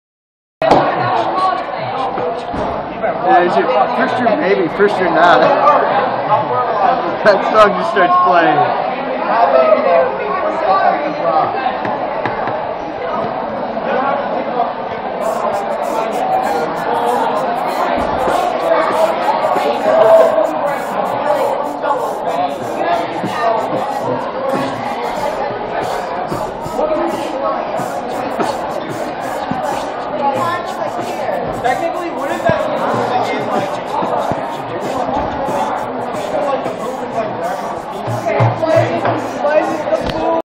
IRL: Lunchtime Boredom
Bell and Jake hammer out some fresh beats!